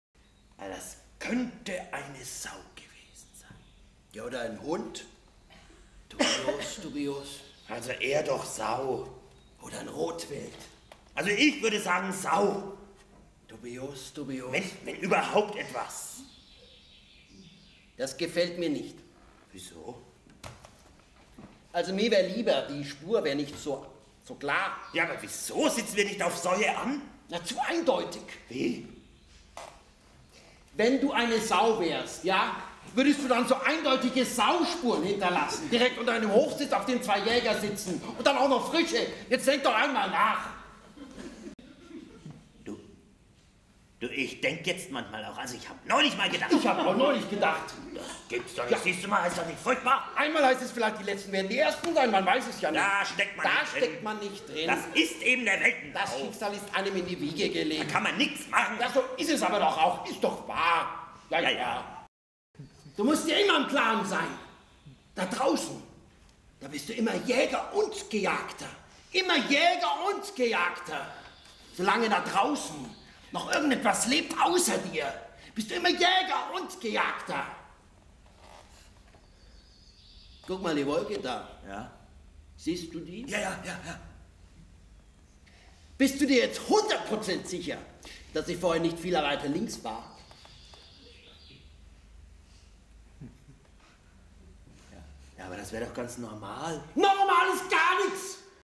Waidmannsheil
Für das Theater4 drehen wir seit zehn Jahren regelmäßig die Vorstellungen im Festsaal des Kunstkulturquartiers in Nürnberg. Im Oktober 2011 kam ein Stück von Susanne Hinkelbein zur Aufführung. Aus dem Stück "Waidmannsheil!" haben wir drei Szenen für die Facebookseite des Theater4 ausgewählt.